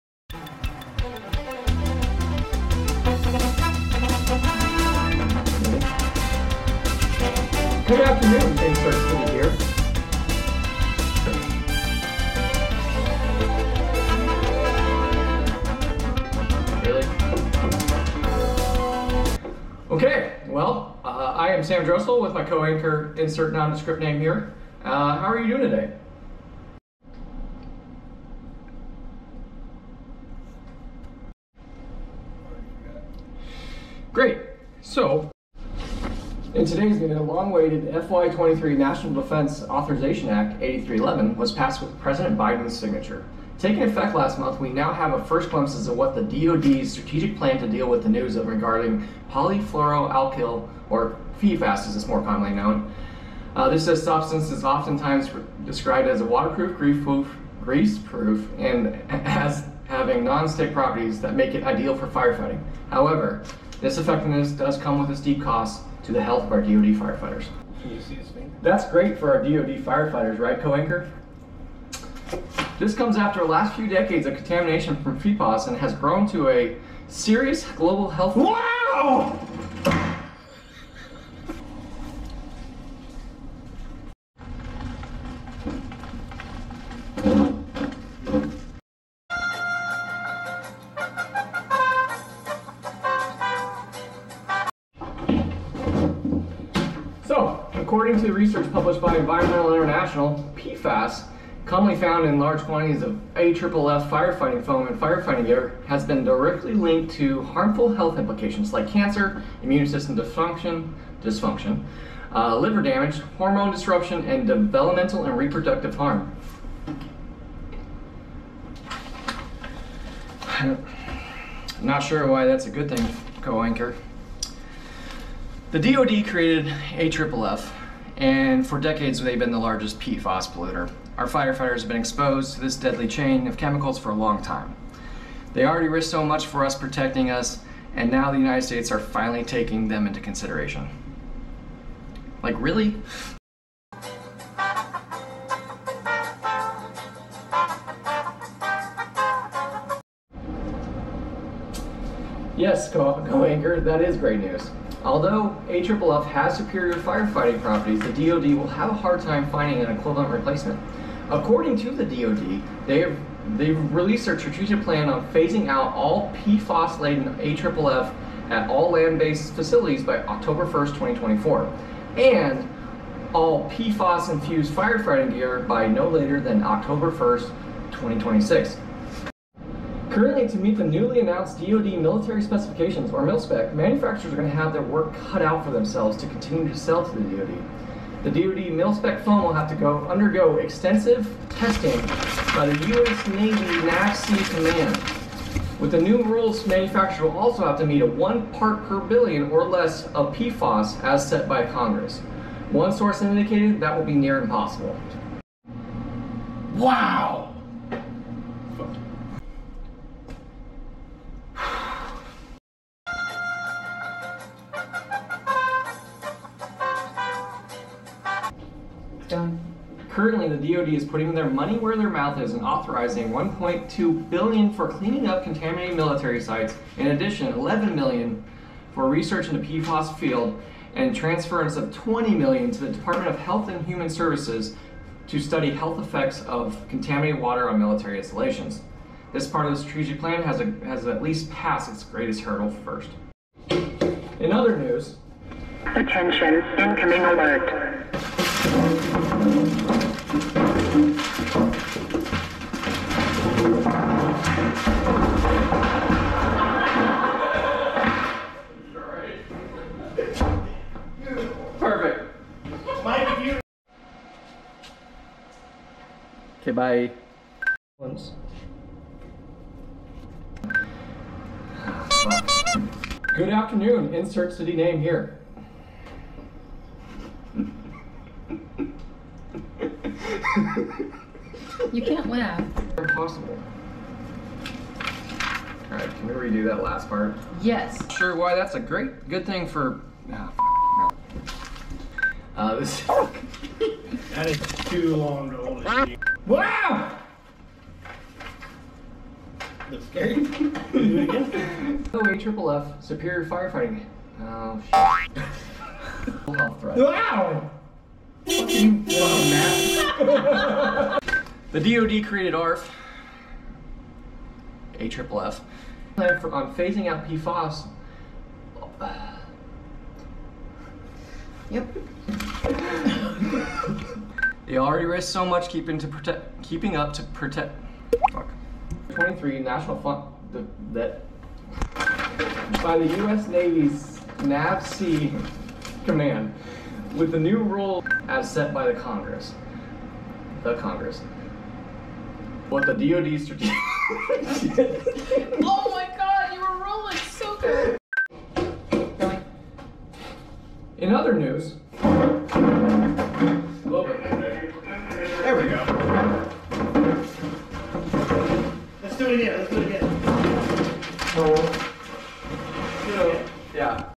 DoD's Strategic Plan to handle PFAS
PFAS is a serious environmental issue that has some equally serious human health implications. The DoD has implemented their strategic plan to start the healing process from decades of contamination.